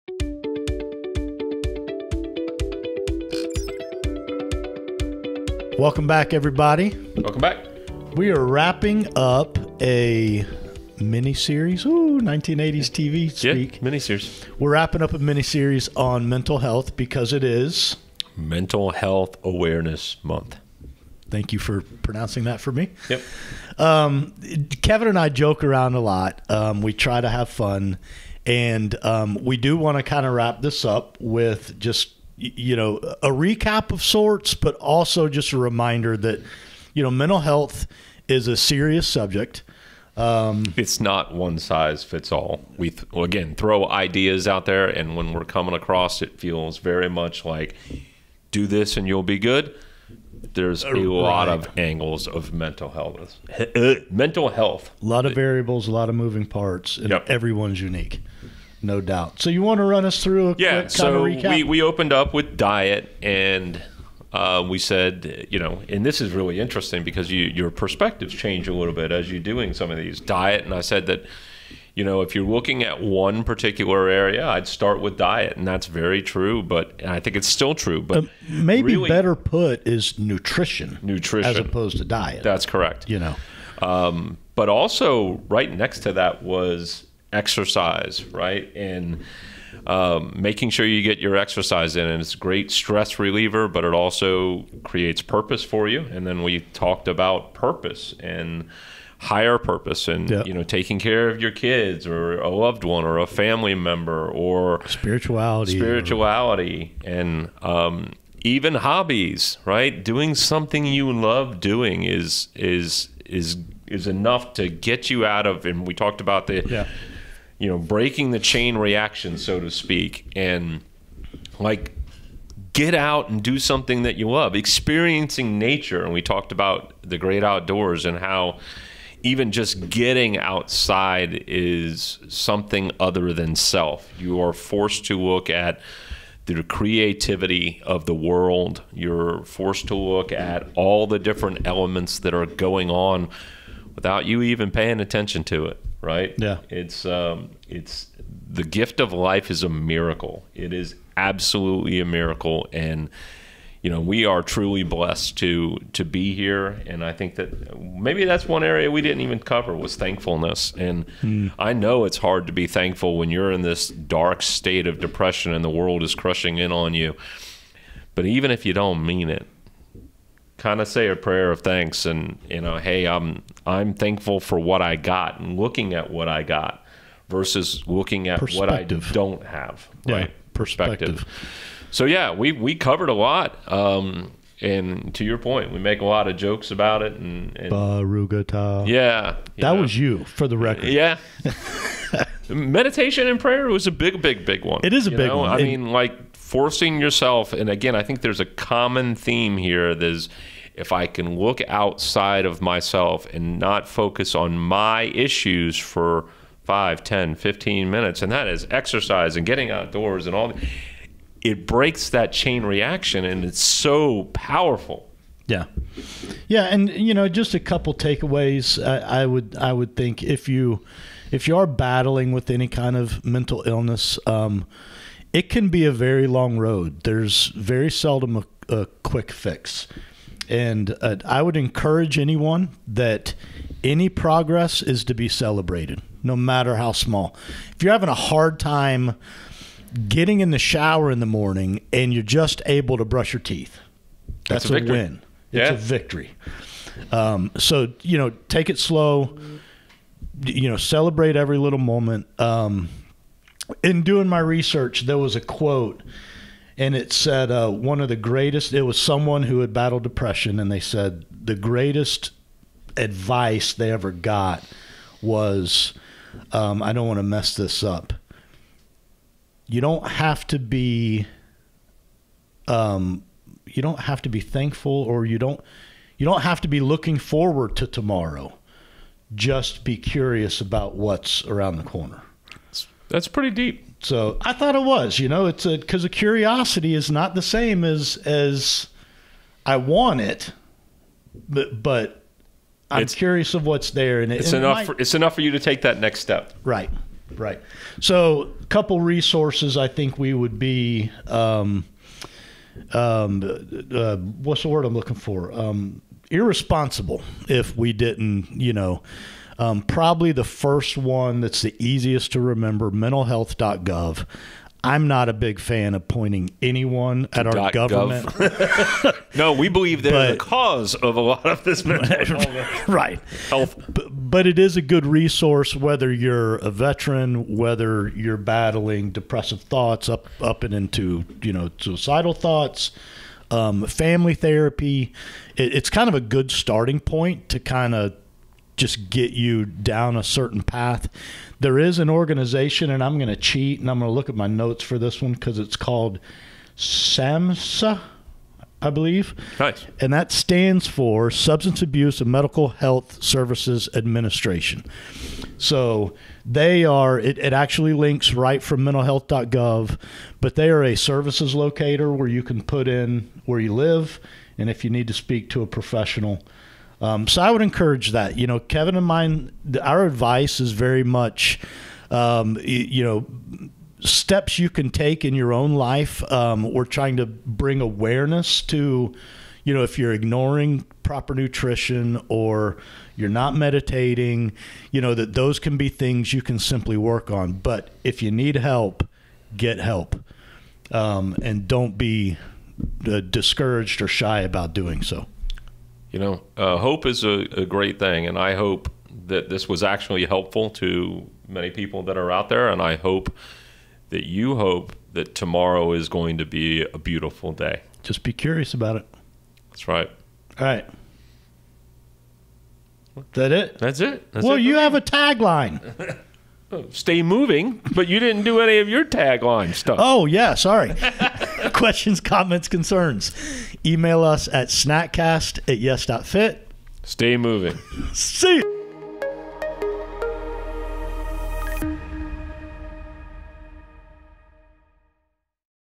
Mental Health Series Part 7 Wrap up- Snack Cast Episode 69
Mental Health is not one size fits all. Wrap up of the Mental Health series. Nutrition, exercise, higher purpose. Being thankful and grateful. Be curious about what tomorrow may bring.